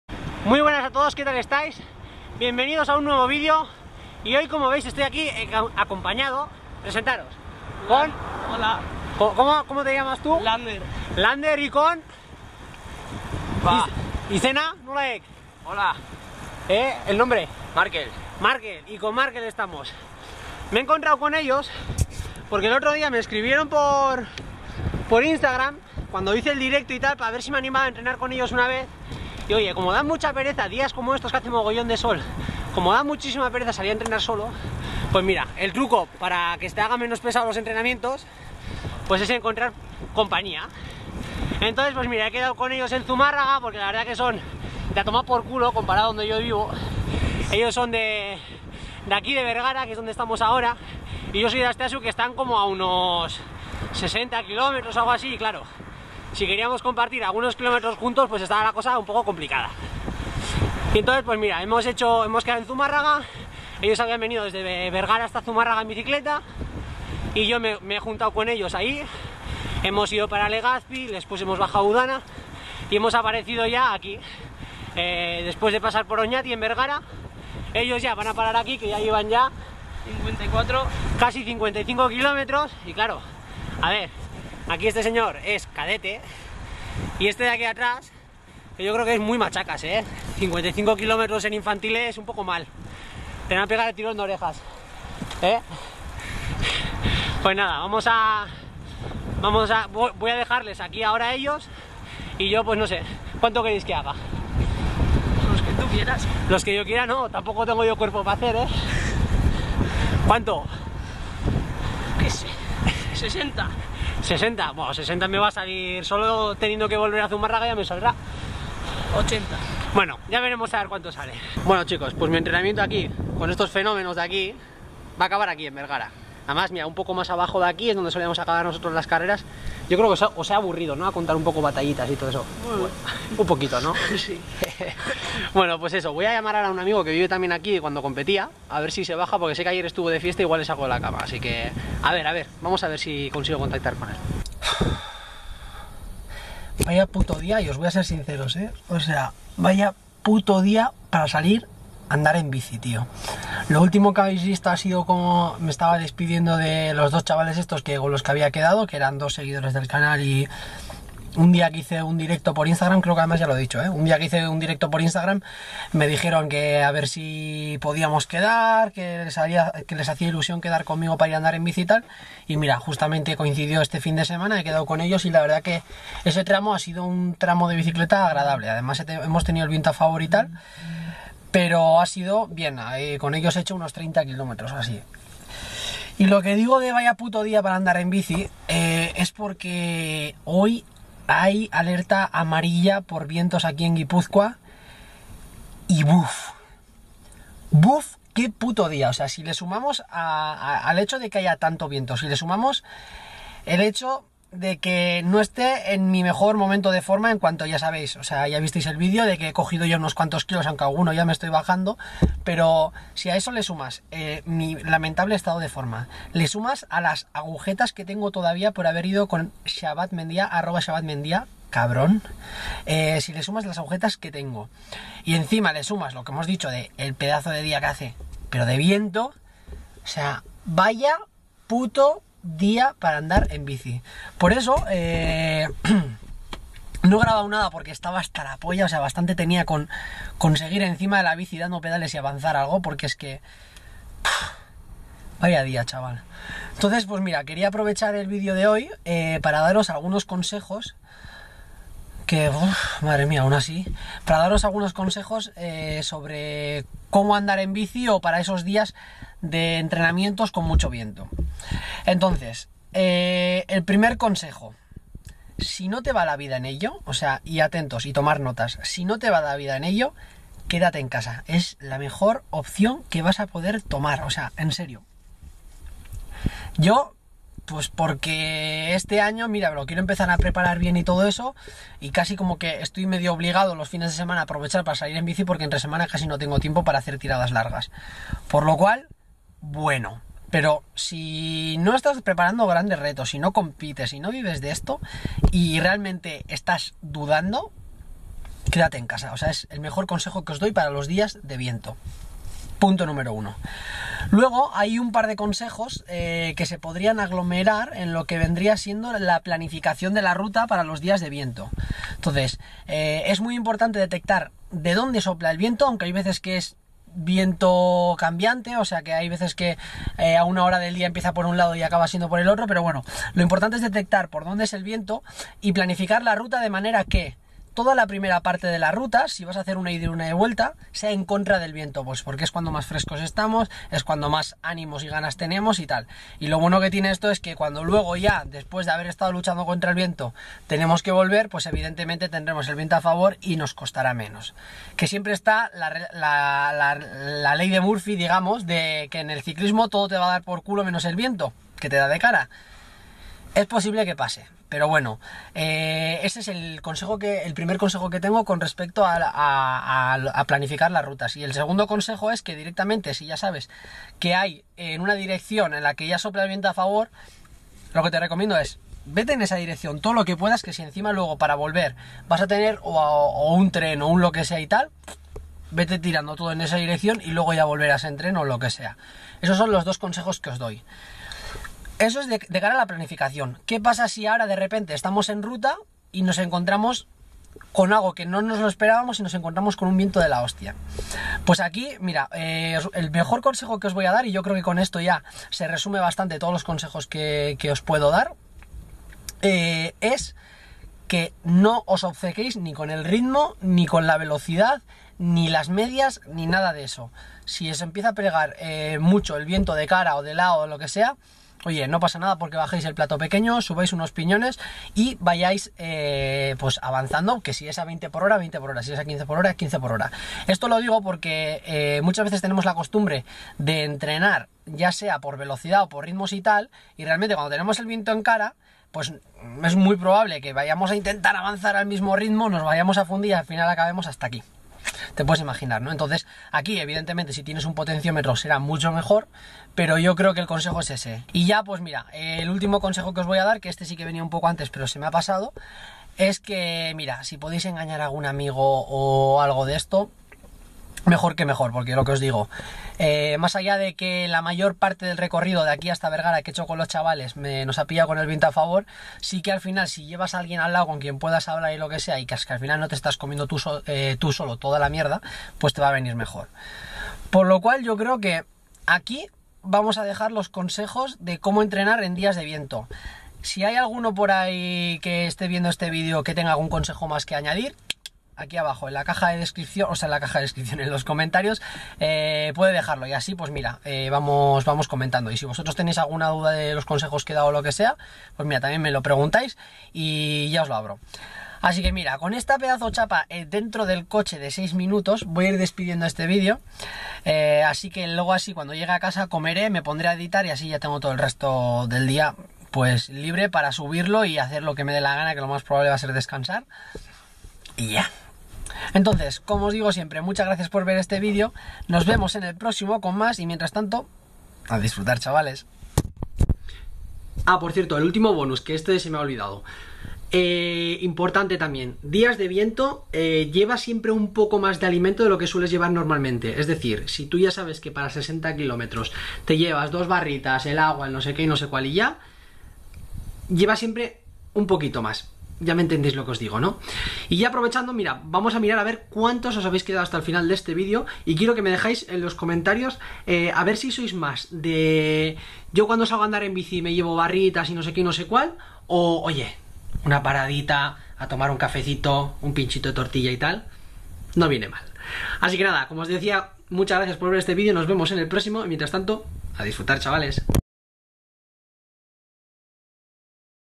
CONSEJOS PARA ENTRENAR CON VIENTO
Seguro que en días como los de hoy, donde había un aviso de alerta amarilla por fuertes vientos en Guipúzcoa te vendrán bien estos consejos. Vaya un puto día que nos ha salido para entrenar con la bici. Toma buena nota de los consejos que aquí te doy para evitarte estas encerronas o al menos sortearlas con mejor pié.